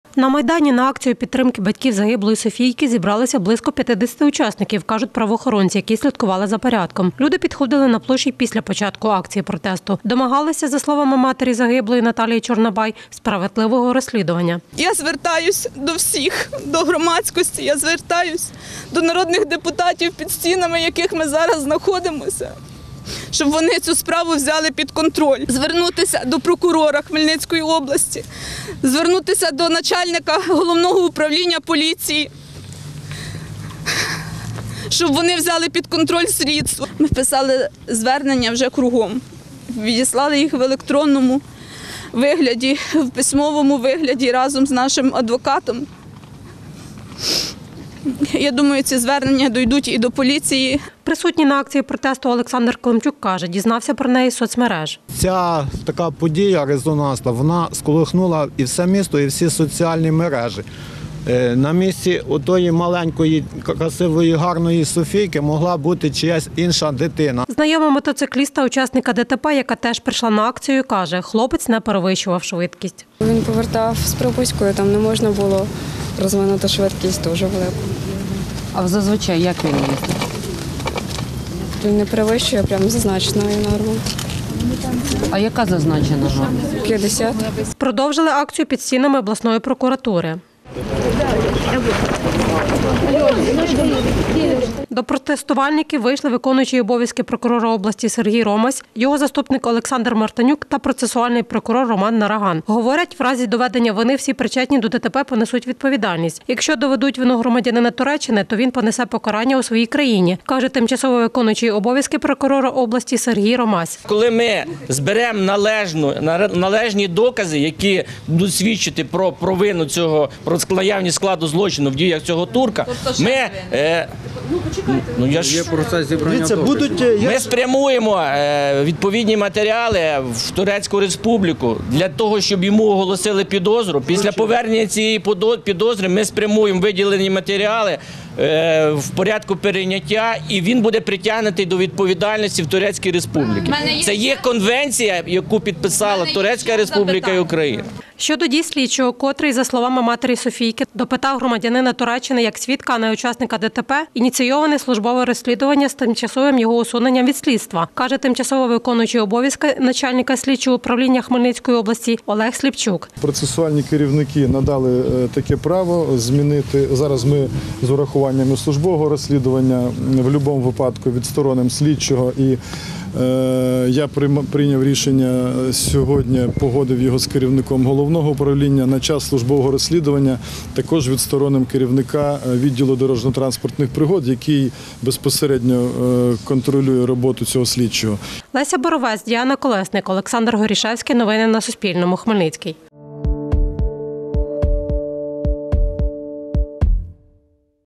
Покарати винних у загибелі 11-річної Софійки Чорнобай внаслідок ДТП вимагали учасники мітингу
Покарати винних у загибелі 11-річної Софійки Чорнобай внаслідок ДТП за участі легковика та мотоцикла вимагали сьогодні учасники мітингу на Майдані  Незалежності в обласному центрі. На площу, за словами організаторки мітингу Наталі Чорнобай, зібралося понад сотня людей. За її словами, водій легковика, громадянин Туреччини, 21 жовтня перетнув кордонон з Румунією. Водій мотоцикла під домашнім арештом